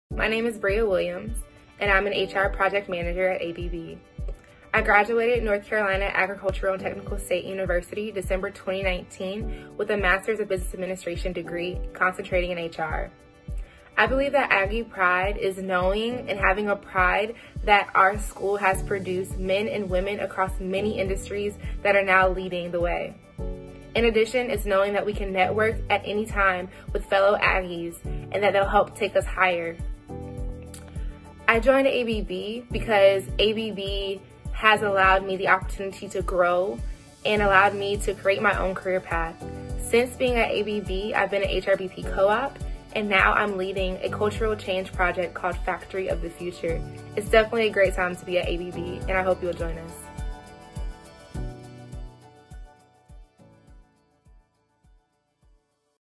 Aggies @ ABB - Bria Williams
Watch Bria Williams, a 2019 graduate of NC A&T, an HBCU in Greensboro, NC show her Aggie Pride! Bria is a member of ABB’s Encompass Black Professionals employee resource group, whose mission is to attract, develop and retain the brightest minds from all walks of life and backgrounds and to build a diverse workforce of leaders. Learn more about diversity & inclusion at ABB